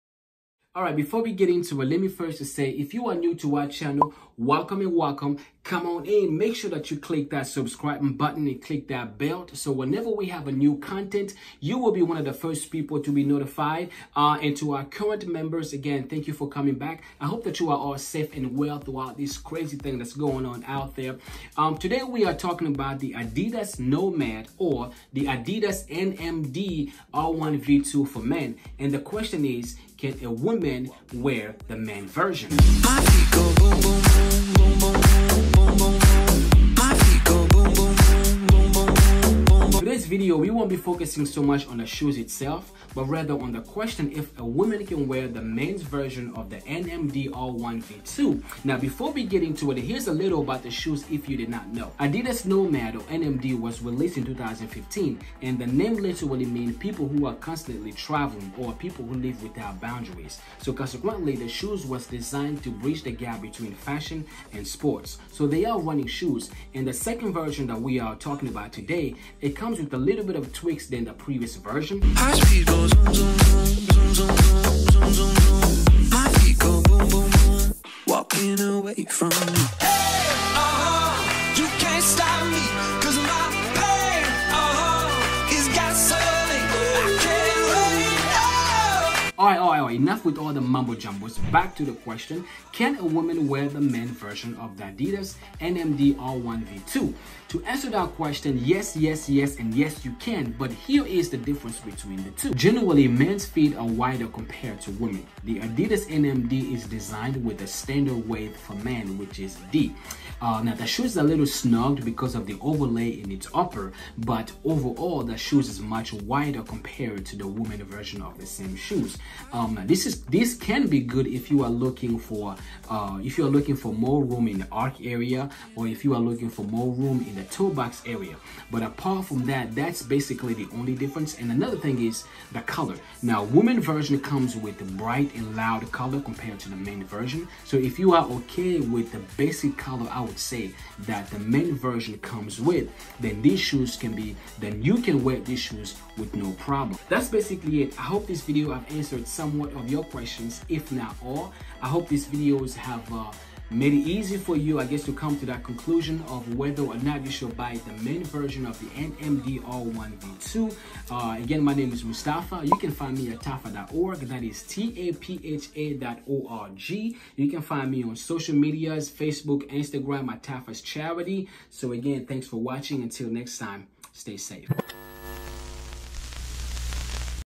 Can a Woman Wear Men’s Adidas NMD_R1 V2 ?
Sometimes it’s a bit difficult to pick the right shoes to fit you comfortably. In this video, I’m answering the question that I’ve been getting quite often about the men’s Adidas NMD_R1 V2. The video is straight to the point. If you have further questions, please feel free to msg me in the comments below.